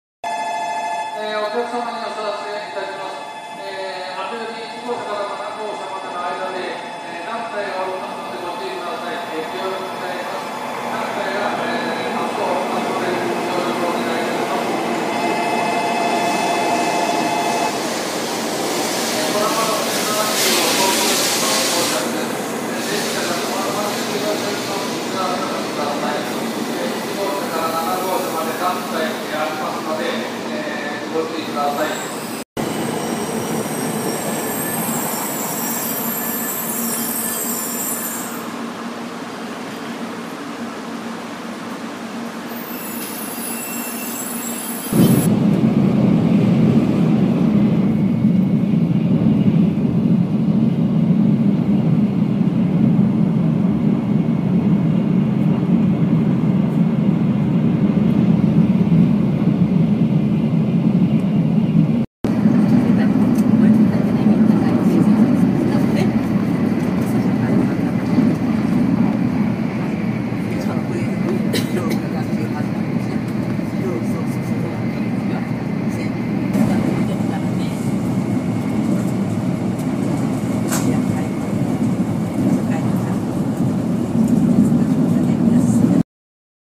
Bullet train in Japan (Shinkansen)
Average speed of the bullet train is 220 км/ч.
Very cool and ultra modern japanese technologies.
Can supply small wholesale lots of cosmetics and
miscellaneous goods from Japan. Negotiable price.